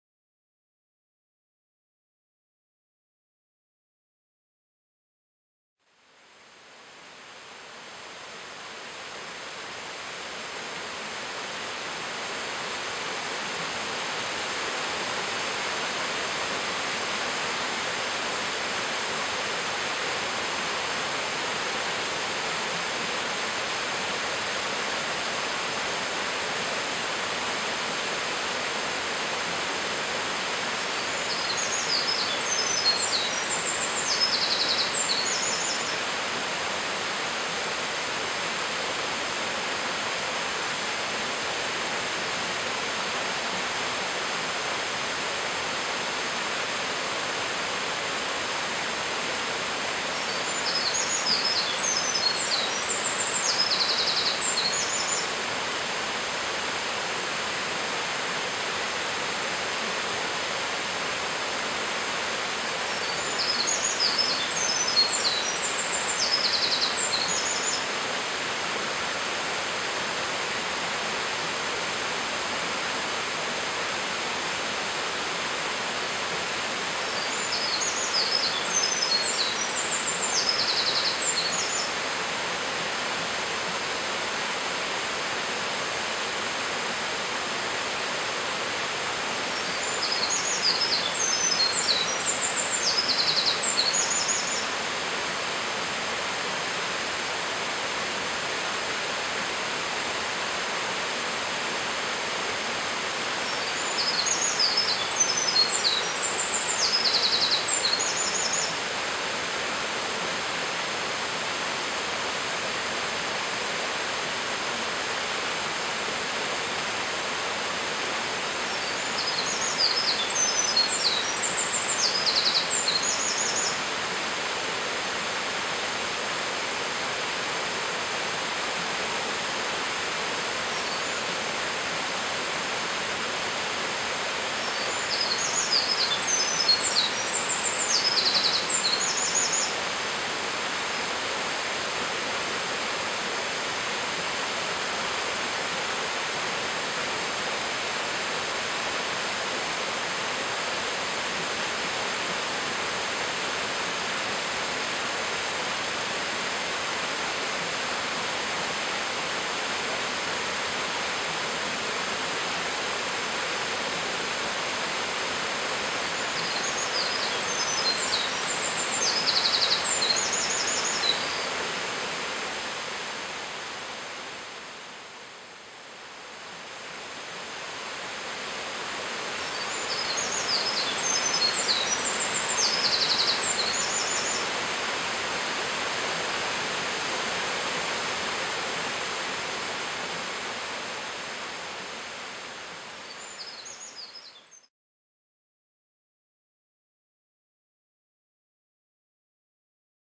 渓流のミソサザイ
2012年6月11日　鳥取県　ミソサザイ滝のすぐ上で囀っていました。